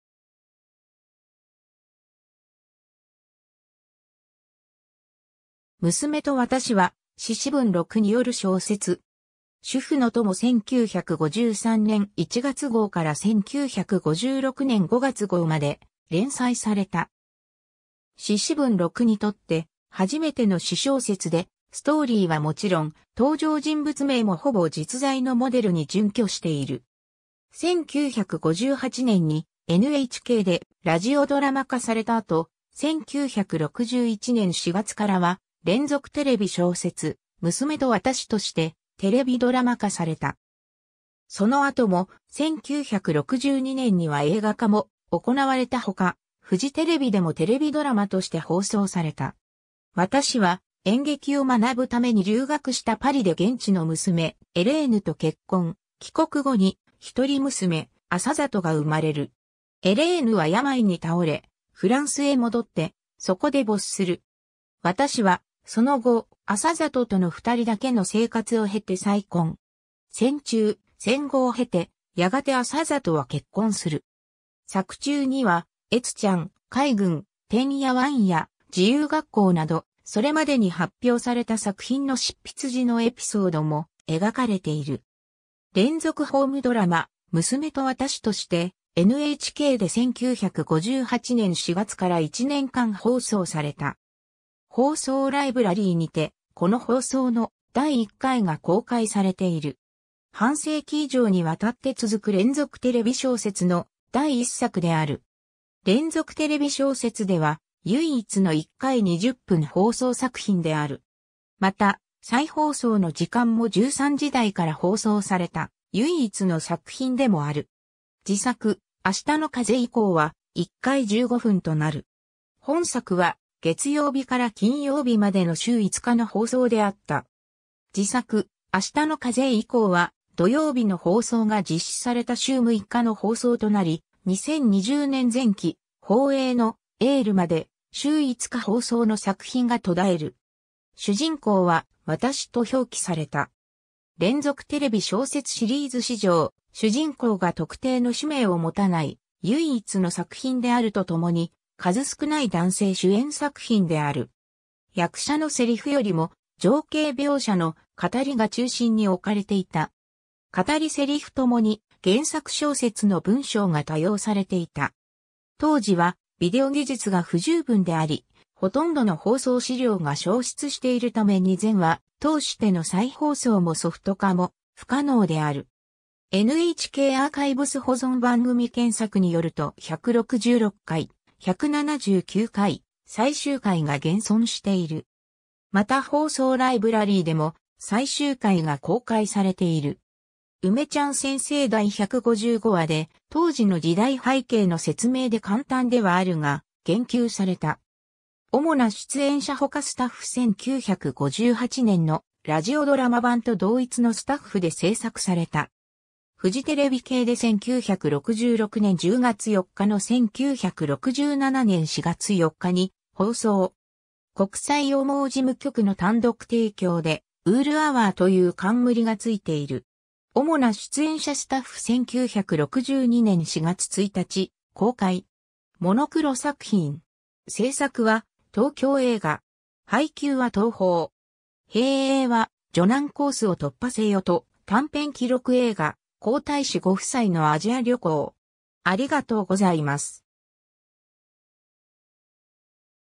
娘と私
日本の自伝・回顧録
『娘と私』（むすめとわたし）は、獅子文六による小説。
 『主婦の友』1953年1月号から1956年5月号まで連載された。
獅子文六にとって初めての私小説で、ストーリーはもちろん、登場人物名もほぼ実在のモデルに準拠している。
1958年にNHKでラジオドラマ化された後、1961年4月からは『連続テレビ小説・娘と私』としてテレビドラマ化された。
その後も、1962年には映画化も行われたほか、フジテレビでもテレビドラマとして放送された。
 「私」は演劇を学ぶために留学したパリで現地の娘・エレーヌと結婚、帰国後に一人娘・麻里が生まれる。
エレーヌは病に倒れ、フランスへ戻ってそこで没する。
「私」はその後、麻里との2人だけの生活を経て再婚。
戦中・戦後を経て、やがて麻里は結婚する。
 作中には『悦ちゃん』『海軍』『てんやわんや』『自由学校』など、それまでに発表された作品の執筆時のエピソードも描かれている。
 『連続ホームドラマ・娘と私』として、NHKで1958年4月から1年間放送された。
放送ライブラリーにて、この放送の第1回が公開されている。
 半世紀以上にわたって続く連続テレビ小説の第1作である。
連続テレビ小説では唯一の1回20分放送作品である（放送時間が、シリーズとしては現時点で最初で最後）。
また、再放送の時間も13時台から放送された唯一の作品でもある（本作は放送時間が朝8:40 - 9:00であった(再放送は13:00 - 13:20)）。
次作『あしたの風』以降は、1回15分となる。
 本作は月曜日から金曜日までの週5日の放送であった。
次作『あしたの風』以降は土曜日の放送が実施された週6日の放送となり、2020年前期放映の『エール』まで週5日放送の作品が途絶える。
 主人公は「私」と表記された。
連続テレビ小説シリーズ史上、主人公が特定の氏名を持たない唯一の作品であるとともに、数少ない男性主演作品である。
役者のセリフよりも情景描写の「語り」が中心におかれていた。
語り・セリフともに、原作小説の文章が多用されていた。
 当時はビデオ技術が不十分であり、ほとんどの放送資料が消失しているために全話通しての再放送もソフト化も不可能である。
NHKアーカイブス保存番組検索によると166回、179回、最終回が現存している。
また放送ライブラリーでも、最終回が公開されている。
 『梅ちゃん先生』（2012年度上半期）第155話で、当時の時代背景の説明で簡単ではあるが言及された。
 主な出演者 ほか スタッフ 1958年のラジオドラマ版と同一のスタッフで製作された。
 フジテレビ系で1966年10月4日 - 1967年4月4日に放送。
「国際羊毛事務局」の単独提供で、「ウール・アワー」という冠が付いている。
 主な出演者（フジテレビ版） スタッフ（フジテレビ版） 1962年4月1日公開。
モノクロ作品。
製作は東京映画。
配給は東宝。
併映は『女難コースを突破せよ』（監督：筧正典。主演：小林桂樹）と、短編記録映画『皇太子ご夫妻のアジア旅行』（構成・編集：渡辺武仁）。